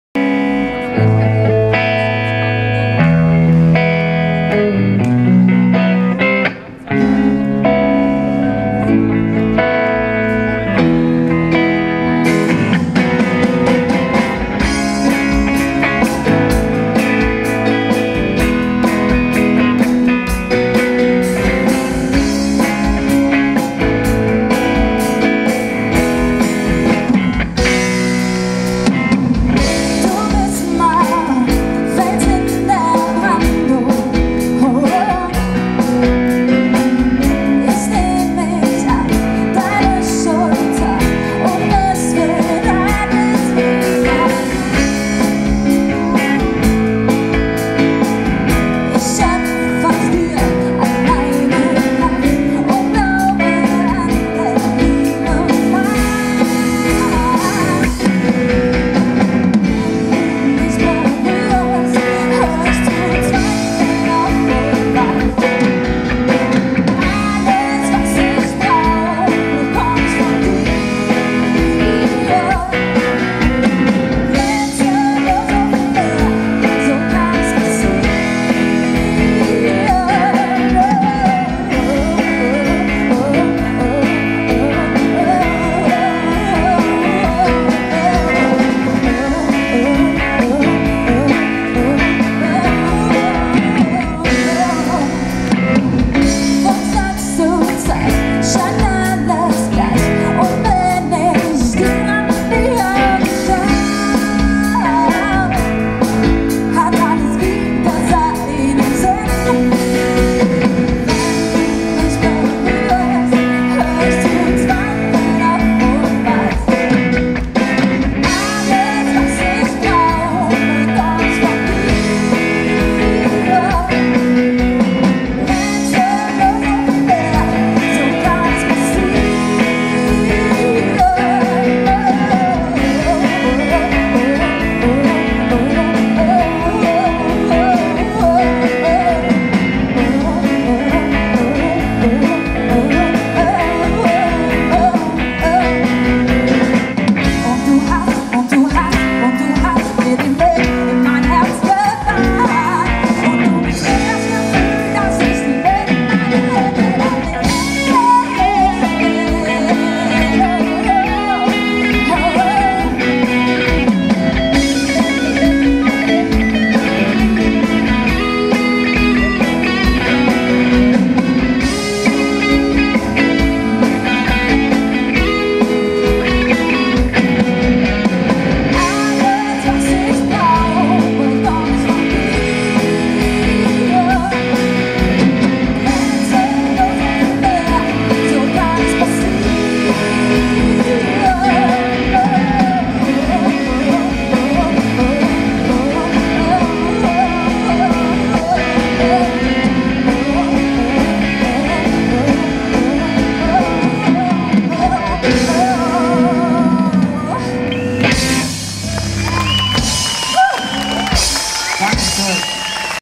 Essen 2012 Original HD Rahmenlos
Rahmenlos bei Essen Original am 2. September auf dem Willy-Brandt-Platz.
Ganz  junge Band, sehr entspannte Musik.